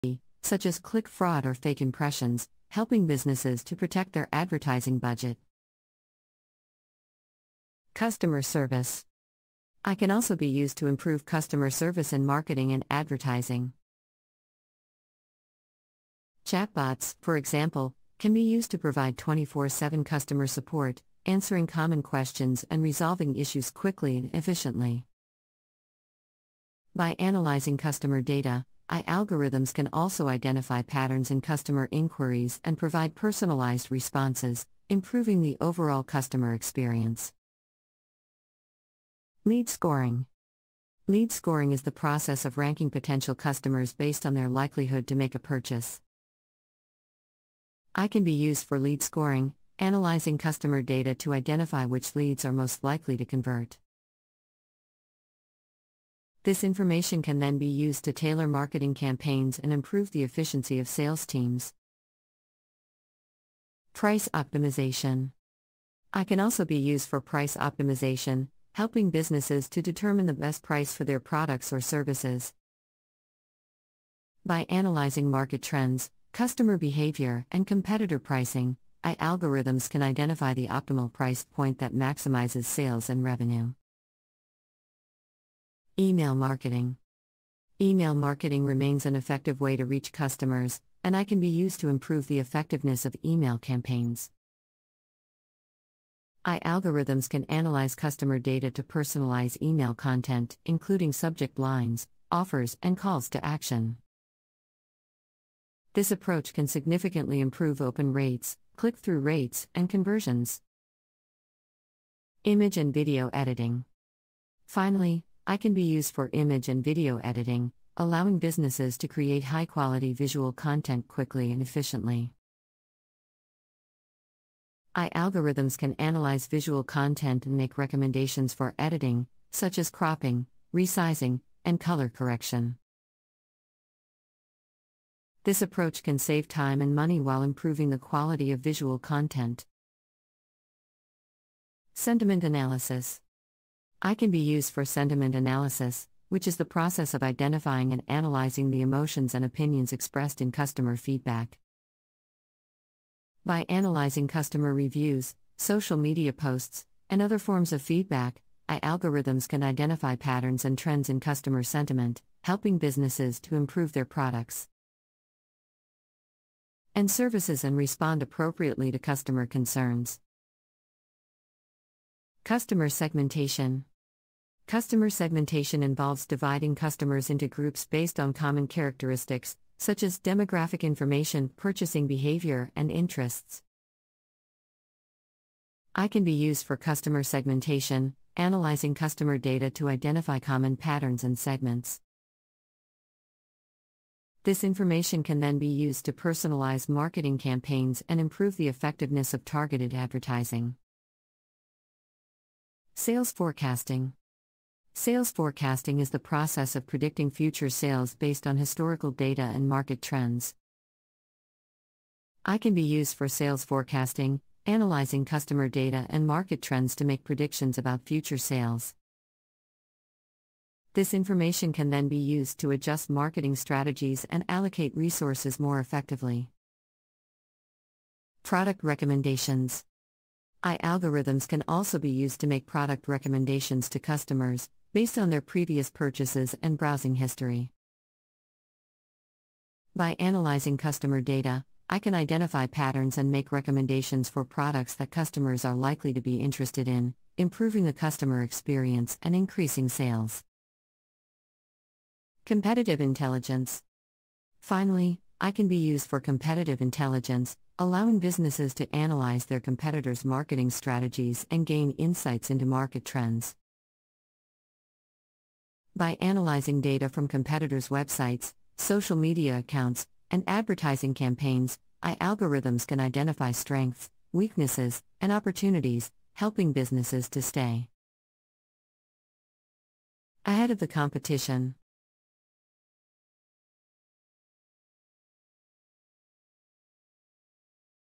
Applications of artificial intelligence in the field of marketing and advertising | Part 2
Artificial intelligence (AI) has been revolutionizing various industries in recent years, including marketing and advertising. With the ability to analyze large amounts of data and make predictions, AI has become a valuable tool for businesses looking to improve their marketing strategies and increase their ROI. In this article, we will explore some of the applications of AI in marketing and advertising.